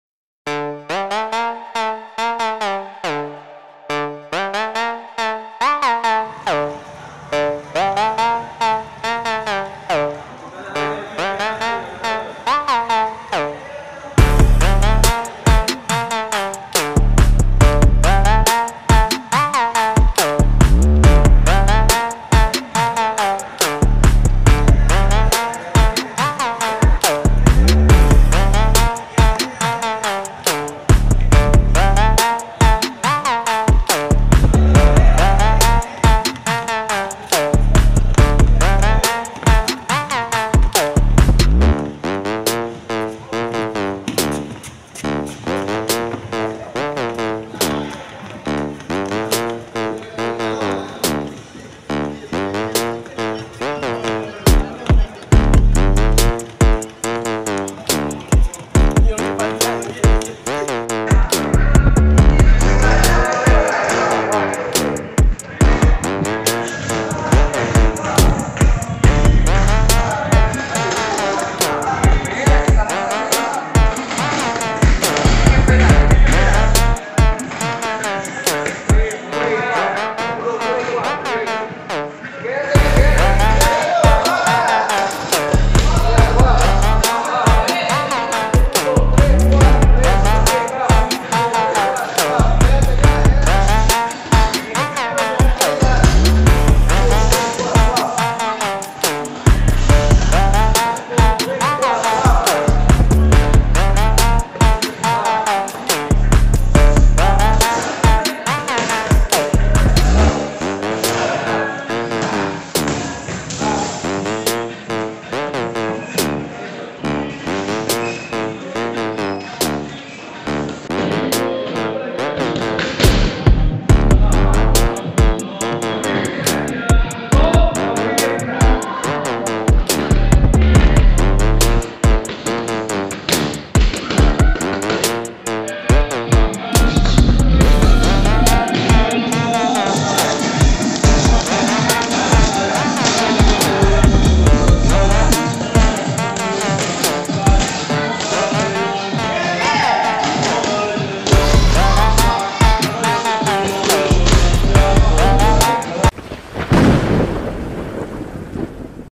Escuela de Boxeo Johnner Gomez 🥊🥊 Coliseo de Boxeo Pedro Gamarro la Cotorrera 🥊🥊
entrenamiento para ponerse a tono 🤼🤼🤼🏋️🏋️🏋️🏋️🥇🥇🥊🥊